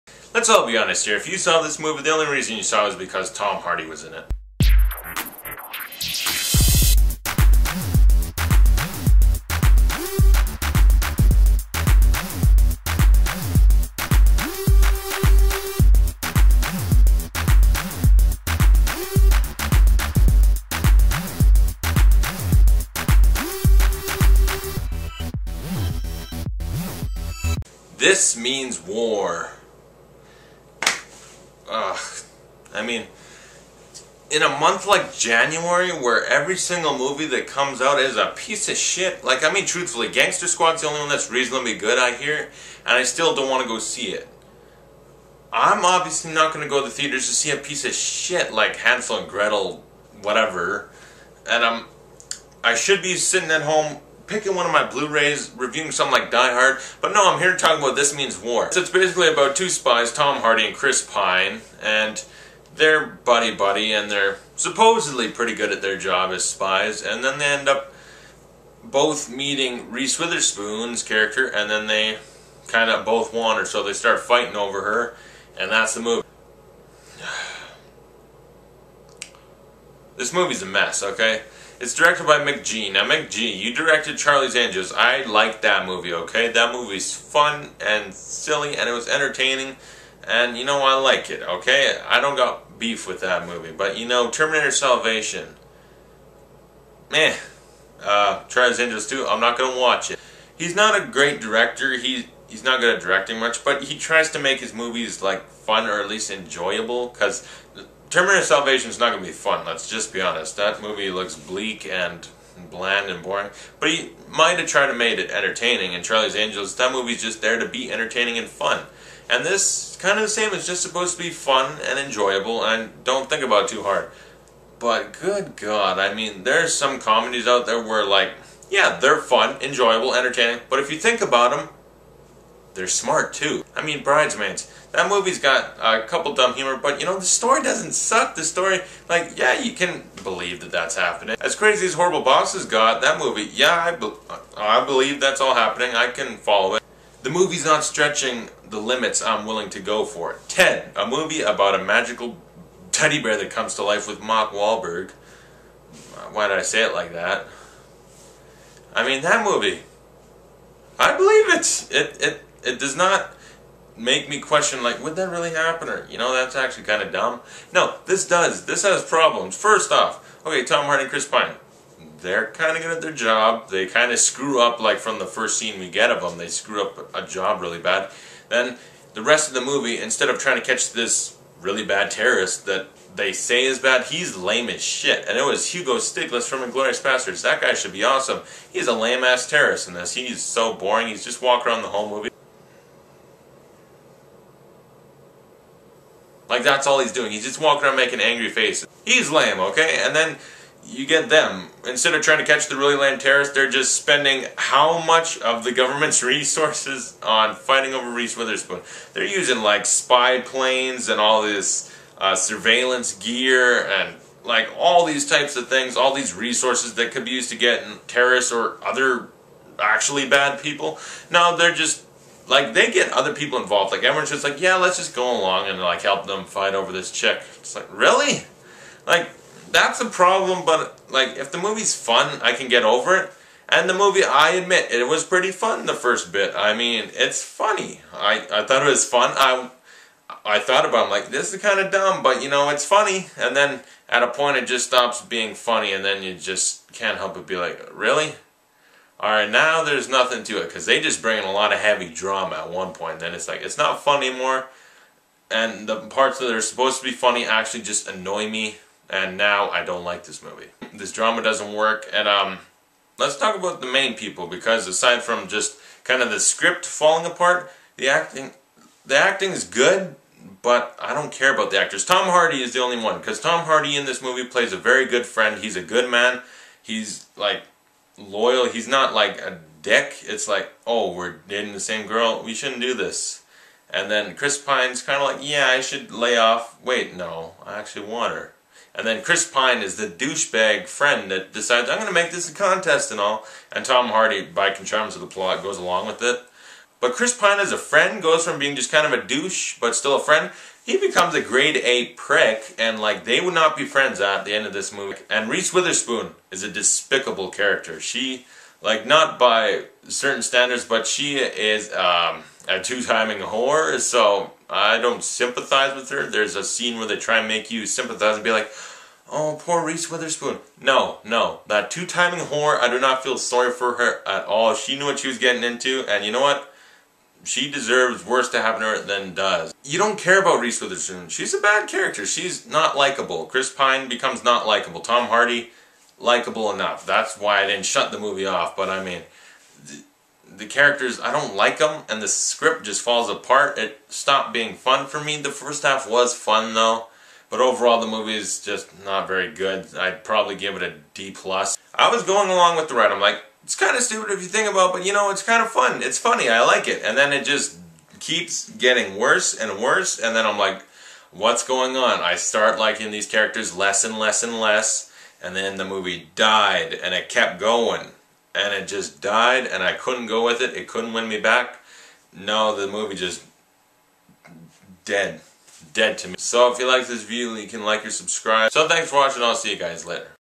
This Means War - Movie Review by Dylan Campbell
I review the comedy starring Tom Hardy, Chris Pine, and Reese Witherspoon. Directed by McG.